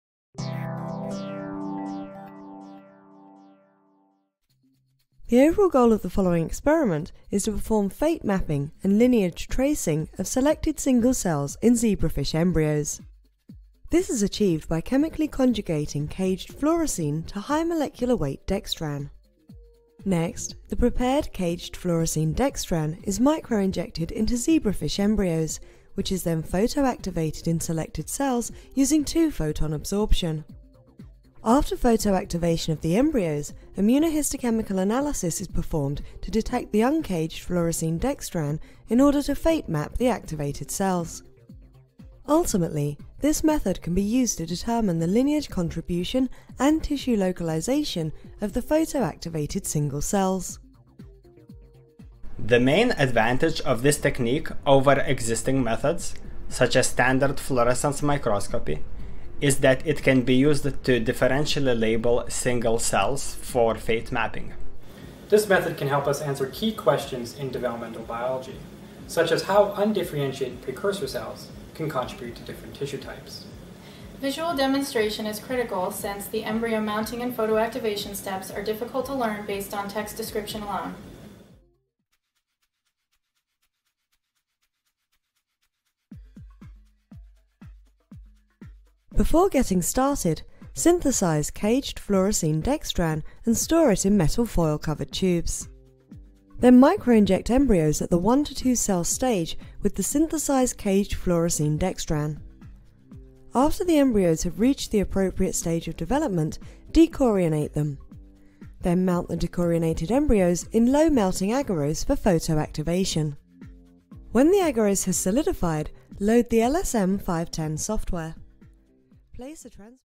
Single Cell Fate Mapping In Zebrafish l Protocol Preview
Single Cell Fate Mapping in Zebrafish -  a 2 minute Preview of the Experimental Protocol

Vikram Kohli, Kira Rehn, Saulius Sumanas
Cincinnati Children's Hospital Medical Center, Division of Developmental Biology; Cincinnati Children's Hospital Medical Center, Division of Hematology/Oncology;

A method is described to photoactivate single cells containing a caged fluorescent protein using two-photon absorption from a Ti:Sapphire femtosecond laser oscillator. To fate map the photoactivated cell, immunohistochemistry is used.  This technique can be applied to any cell type.